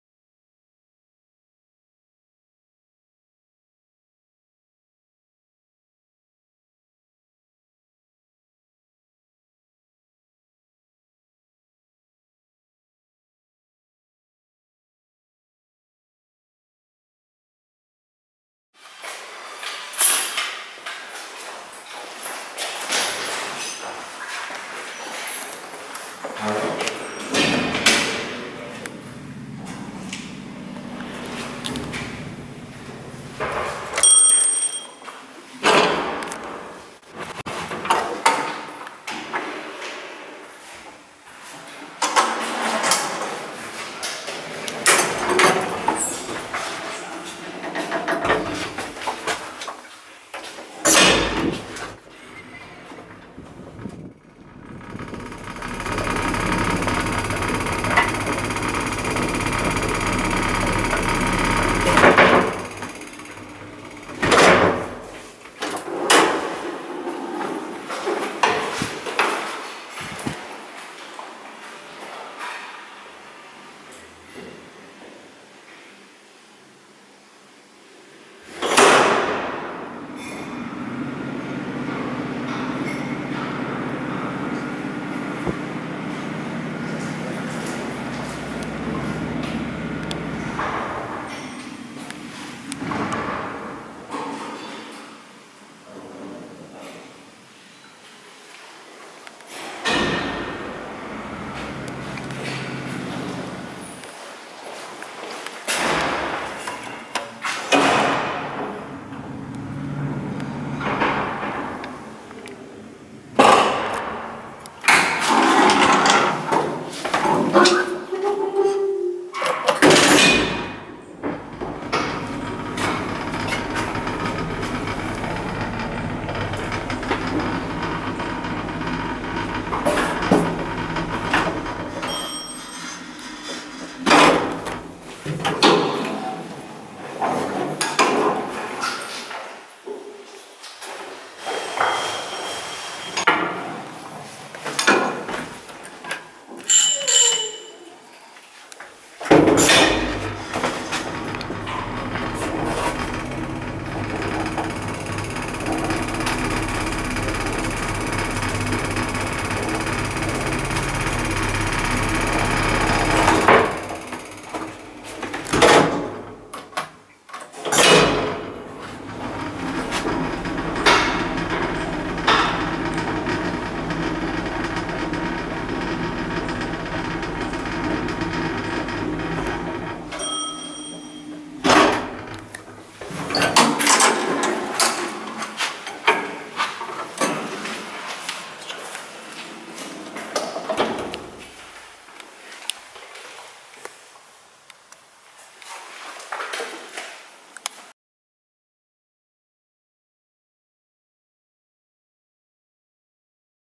Winda ZREMB w hotelu robotniczym @Witosa 32, Opole
»Firma: ZREMB
»Rok produkcji: 1987
»Udźwig: 500kg/6 osób
»Obsługiwane kondygnacje: 6 (S,P,...,4)

Nagrania i zdjęcia:
- Sony Cyber-Shot HX60v